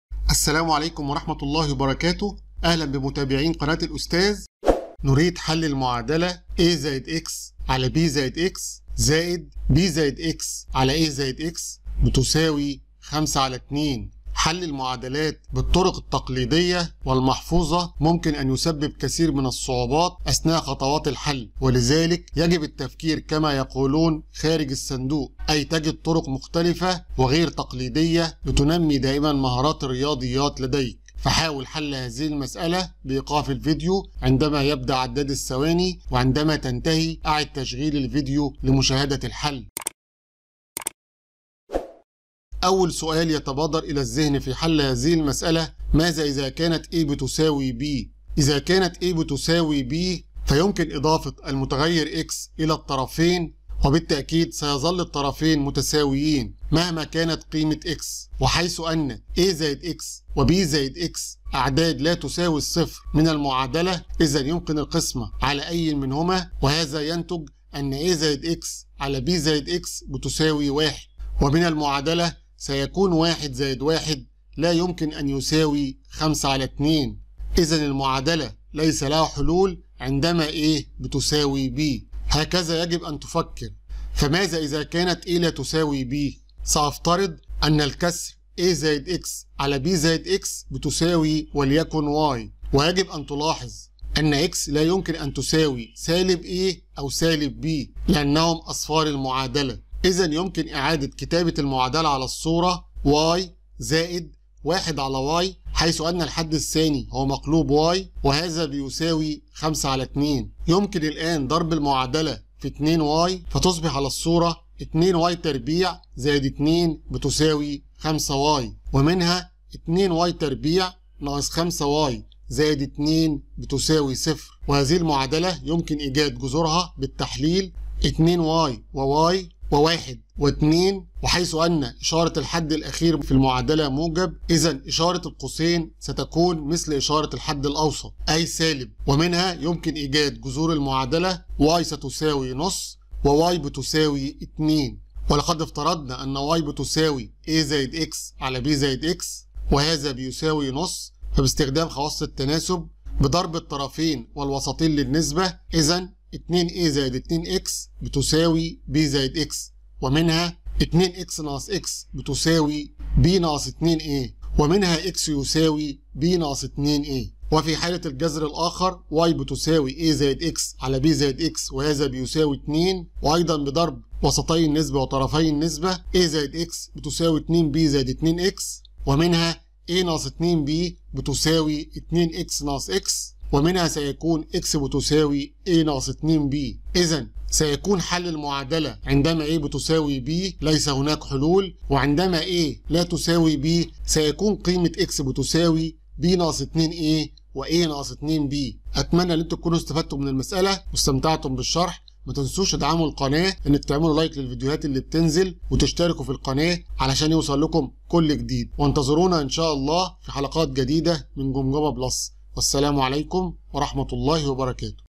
هل تستطيع إيجاد جميع حلول المعادلة الموجودة أعلاه ؟
كيفية استخدام طرق حل غير تقليدية ( خارج الصندوق )

سلسلة جمجمة ( بلس ) - حلقة (69)
قائمة فيديوهات سلسلة جمجمة ( بلس )

00:01 المقدمة
00:05 عرض المسألة : كيفية إيجاد حلول هذه المعادلة دون استخدام طرق تقليدية
00:40 عداد الثواني ( يمكن وقف الفيديو - للتفكير قليلا في الحل )
00:43 شرح حل المسألة بالتفصيل
03:19 خاتمة الفيديو

♦ يسعدنا اشتراكك معنا في القناة لمتابعة كل جديد
▼تابعنا على وسائل التواصل الاجتماعي